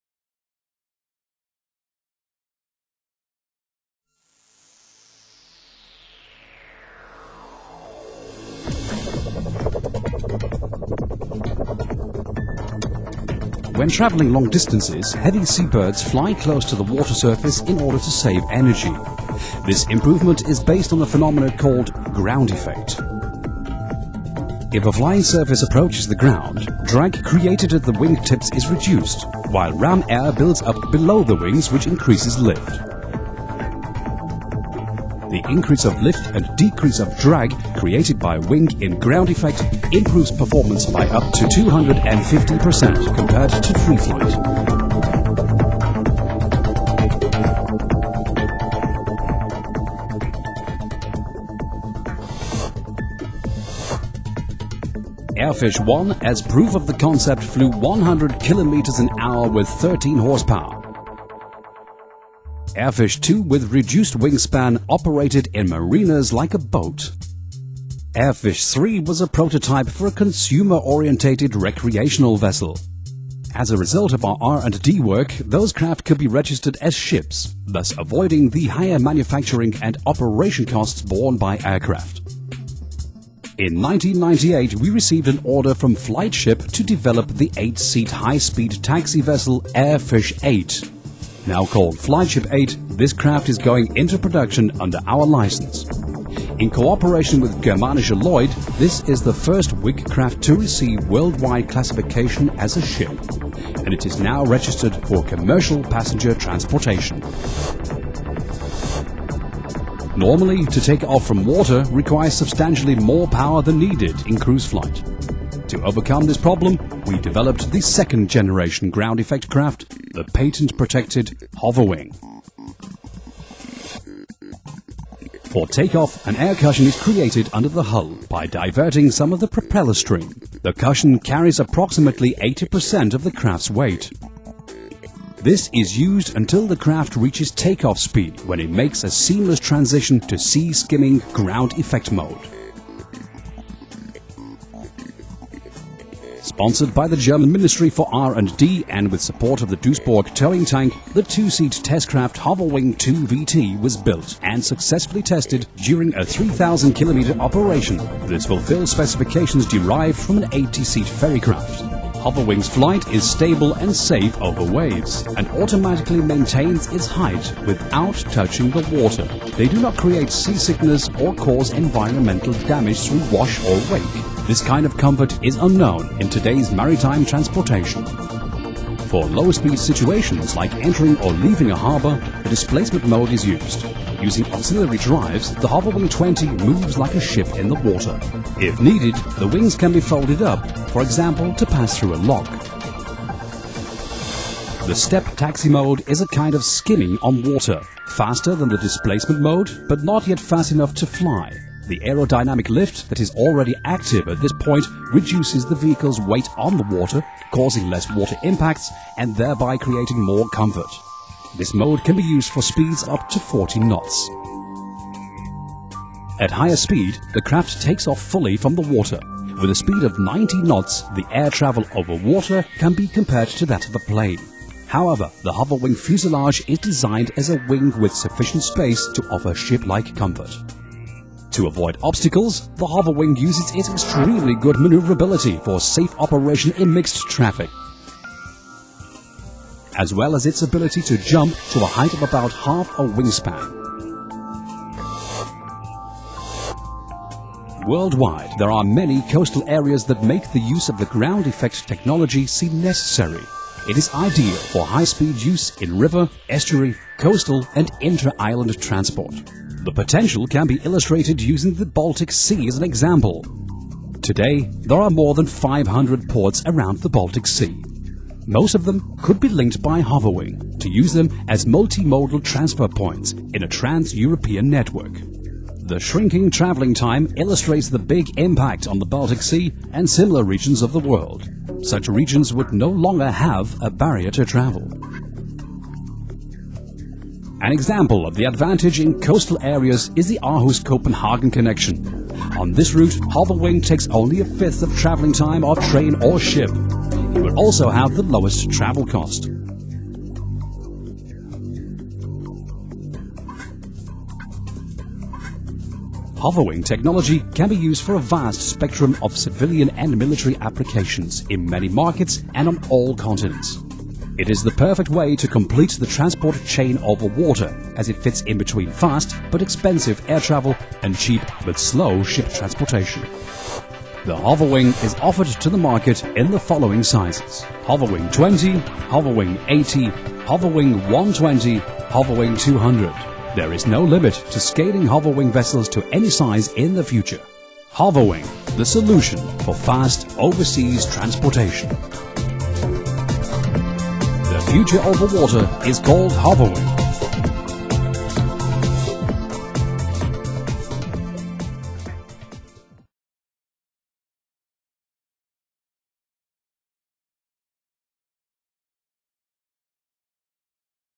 WIG - Wing in Ground Effect
A Wing In Ground-effect vehicle (WIG), is a vehicle that cruises little more than a few feet over flat surfaces, most often water. It can be seen as a transition between a hovercraft and an aircraft. As it flies just above a surface, it is also classified as WISE or WISES (Wing In Surface Effect Ship). WIGs are also classified as Ground Effect Vehicles, or Sea Skimmers. The IMO has classifed the WIGs as ship, with effect of (GEV).[1] In conventional aircraft, "flare" is the rotation of the aircraft's nose up, used at the final part of landing to arrest the descent rate before touch down. In WIG vehicles, this is the basic attitude to sustain level flight.